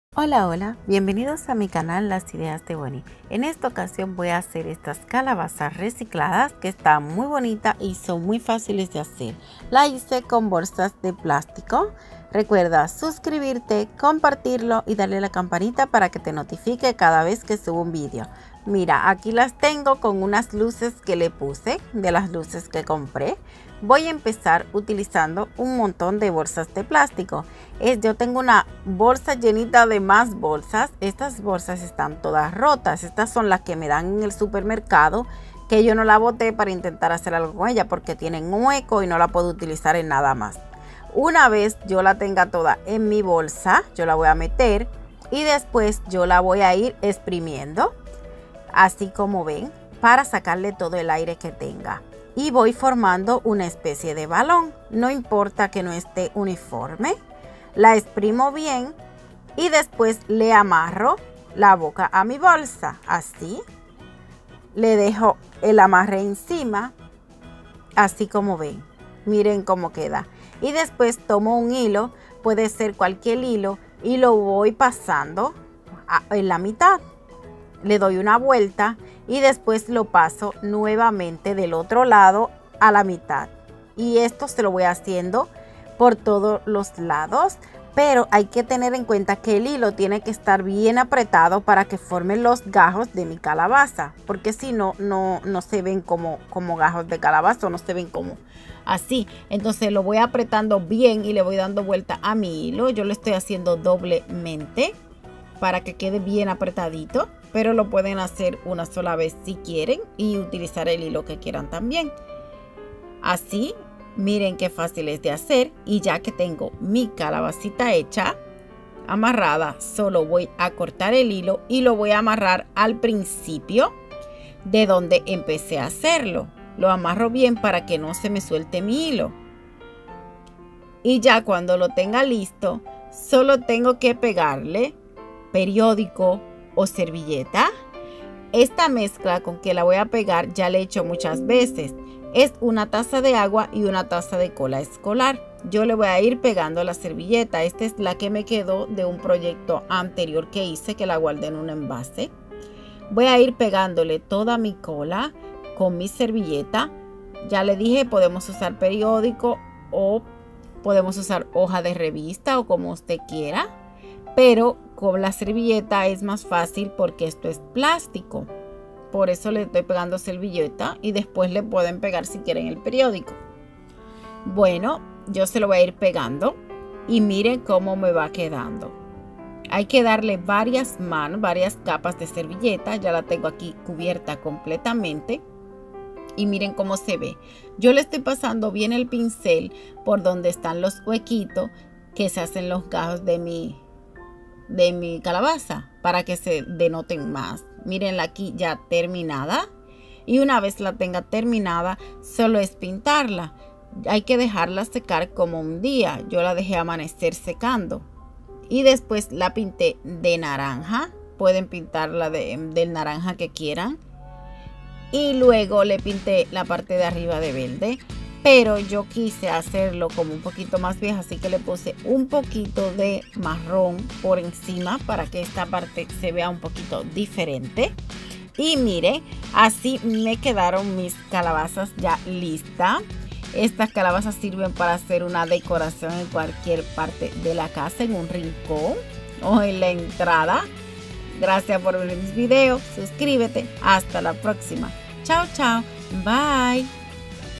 como hacer calabazas con reciclaje calabazas hechas de bolsas de plastico...
HOLA bienvenidos  a mi canal las ideas de bony en el video tutorial de hoy les enseñare como hacer una calabaza con materiales reciclados como lo son las bolsas rotas de los supermercados esta calabaza esta super bonita y es muy facil de hacer y esta genial para decorar un rincon de la casa. pumkin